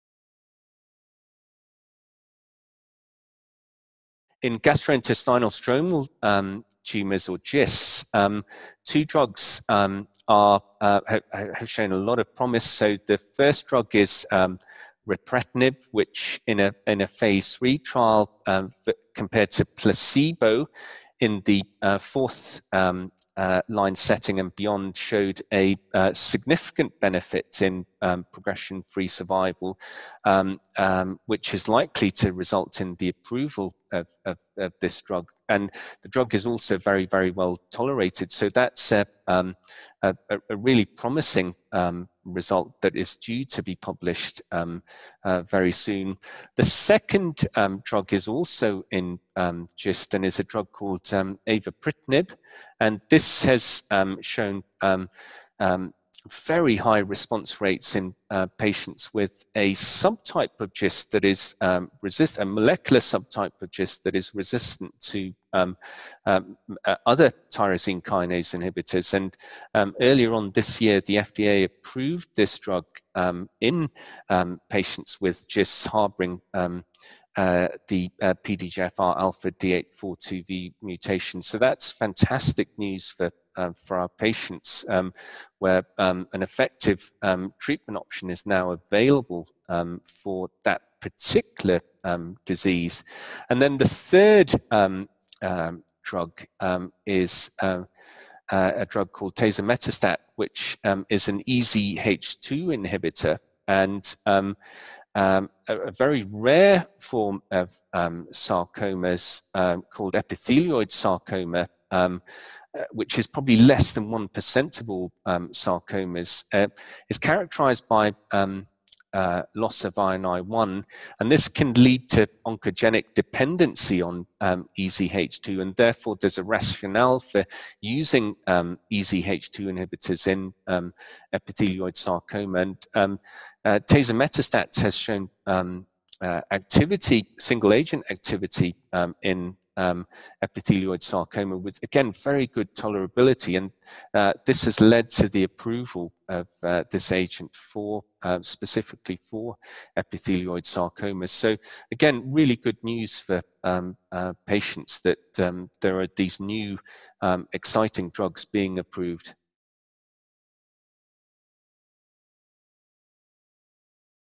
New therapies in soft tissue sarcomas
Robin Jones, MBBS, MRCP, MD (Res), Royal Marsden NHS Foundation Trust, London, UK, highlights the latest updates on new therapies in soft tissue sarcomas. This interview was recorded via an online conference call with The Video Journal of Oncology (VJOncology).